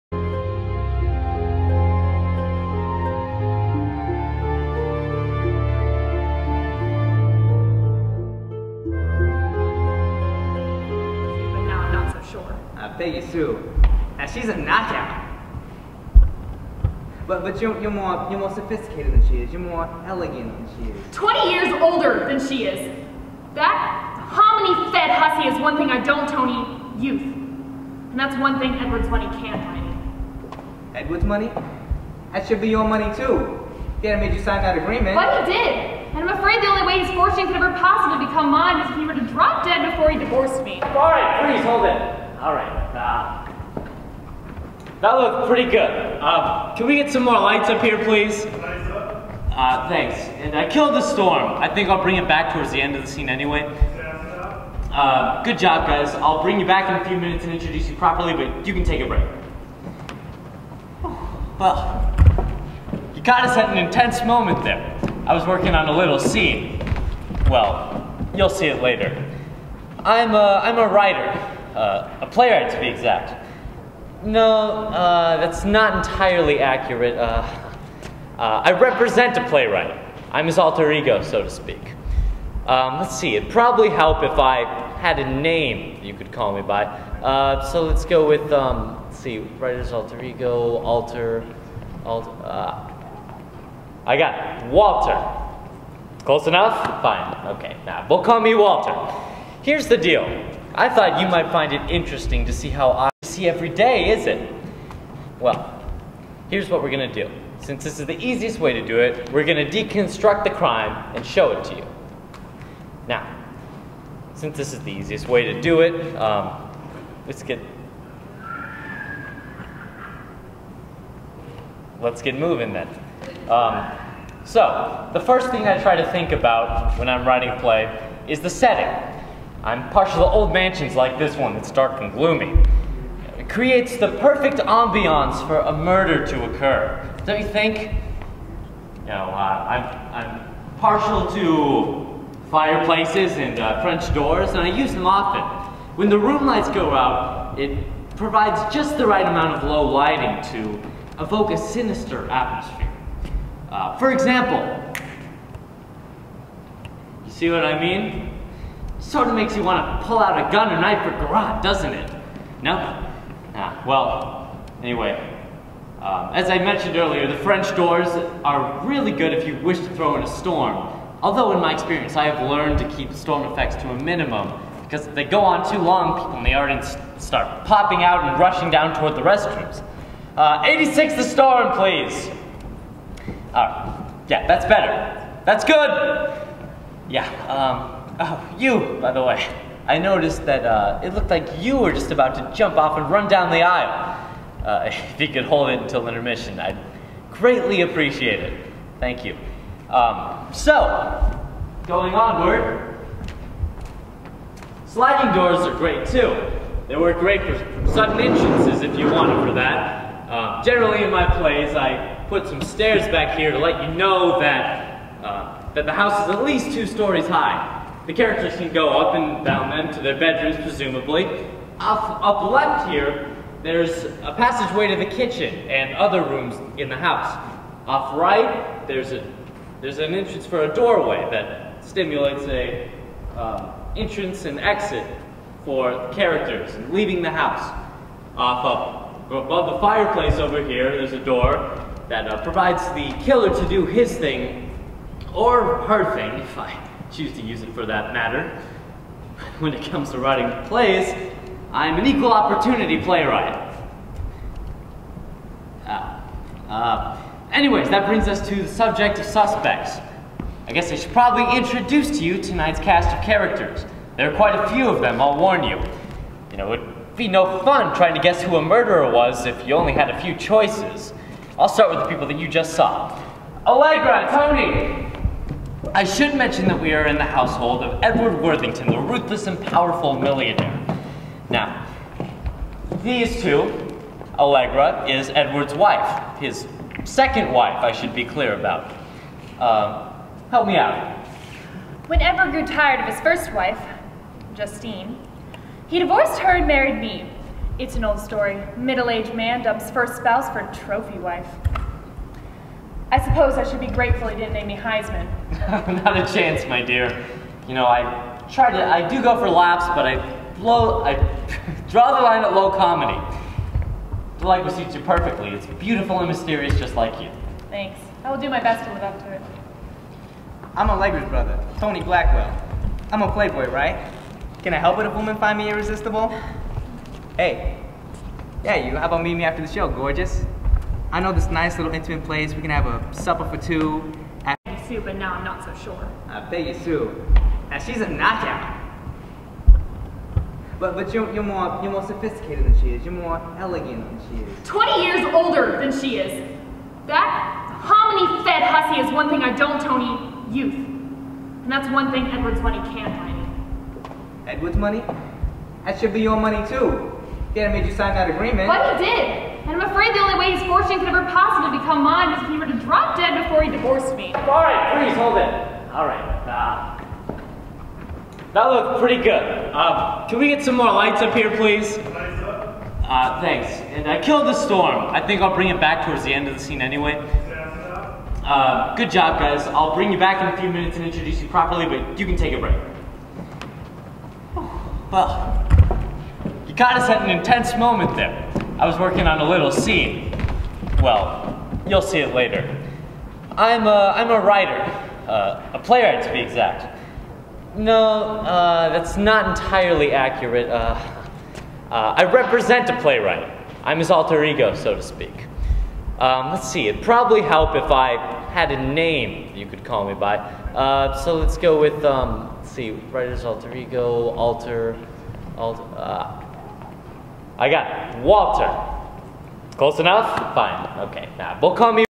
"The Plot, Like Gravy, Thickens" Rehearsals
CAST!
Sorry for the crappy footage! This is not the entire play (only scenes). The footage was first intended for a 1 minute filler for PadreTV. But Mr. McChesney suggested to post this on here because it might be helpful to some of you.